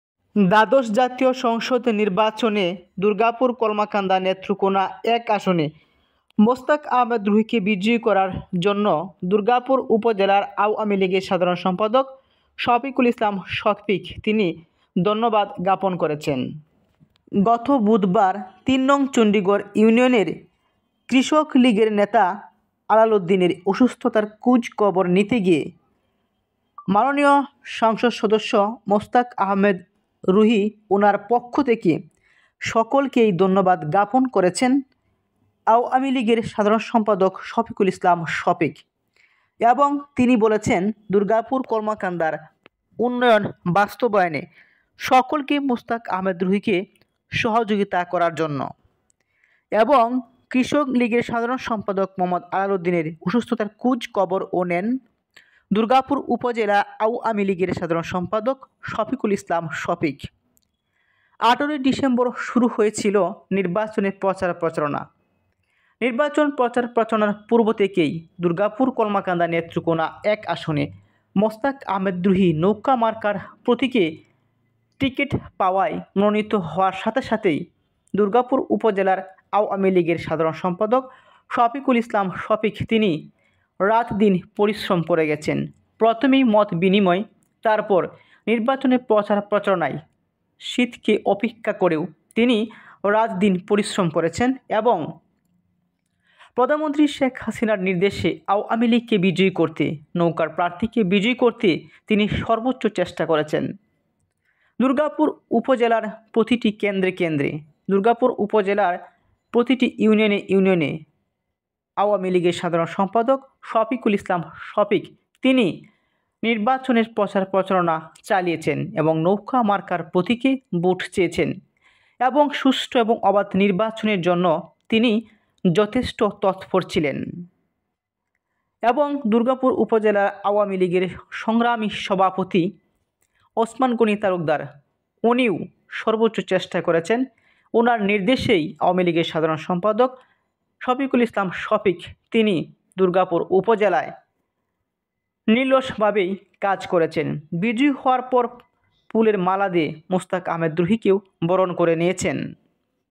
রুহীকে বিজয়ী করার জন্য ধন্যবাদ জানিয়েছেন শফিকুল ইসলাম শফিক
নেত্রকোনা ১ মুস্তাক আহমেদ রুহিকে বিজয়ী করার জন্য ধন্যবাদ জানিয়েছেন আ.লীগর সা.সম্পা শফিক

All videos of the page are created by the admin of the page. This page always creates videos according to Facebook rules.